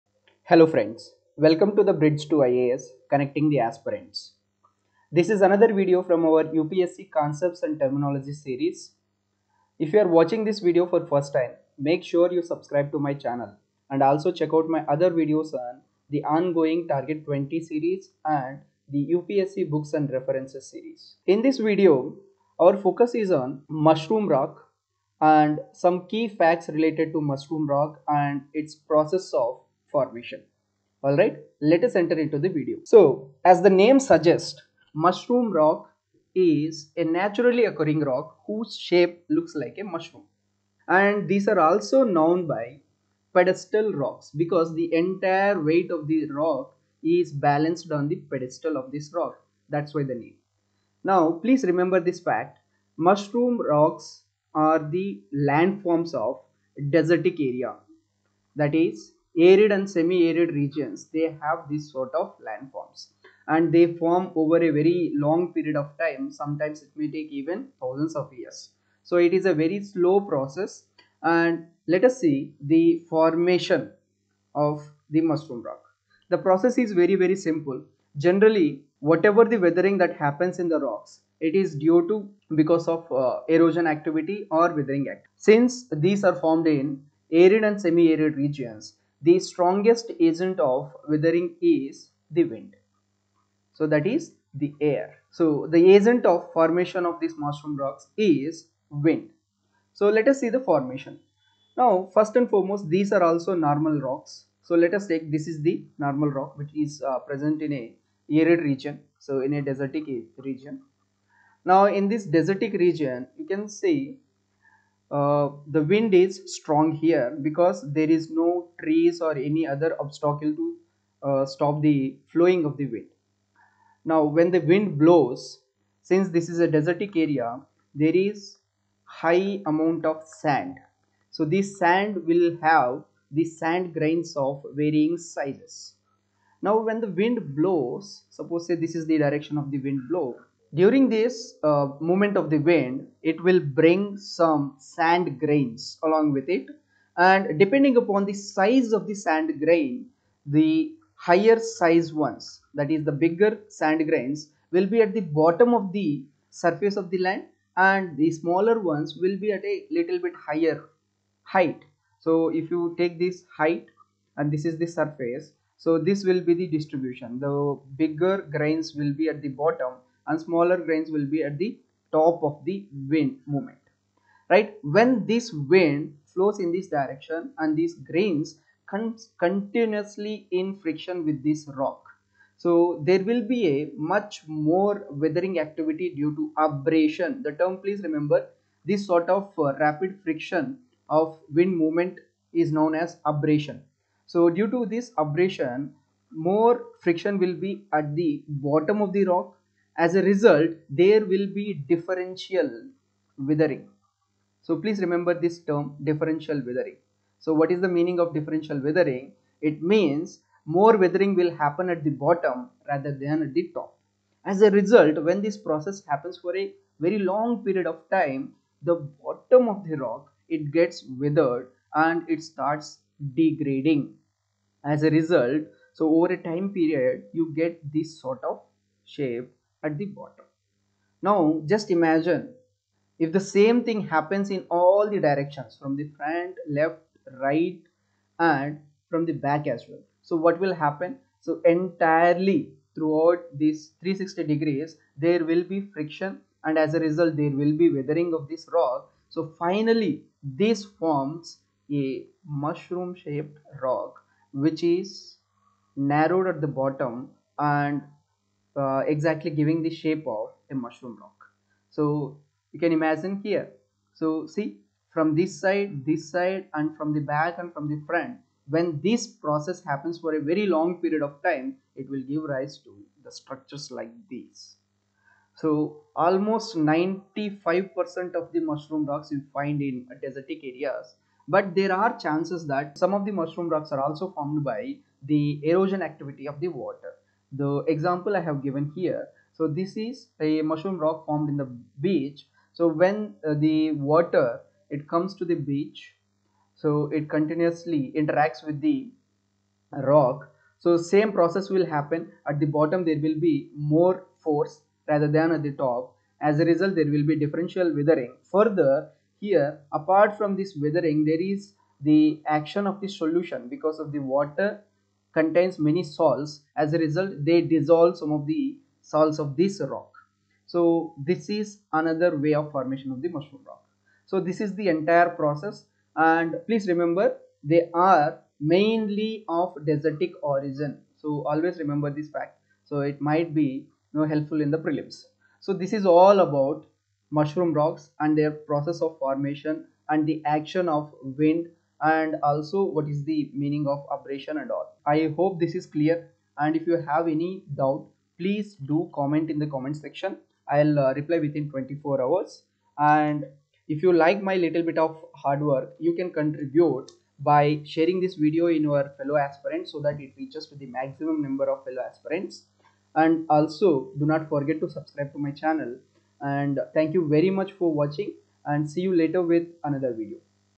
CRACK UPSC | CONCEPTS AND TERMS | MUSHROOM ROCK
Hi friends... This is another video from concepts and terminology of UPSC series. In this video i have explained about Mushroom Rock and Its formation.The video also covers many key facts with respect to it, enjoy the video.

UPSC 2020-21, UPSC prelims startegy, How to prepare for prelims, How to improve prelims score, important topics for prelims.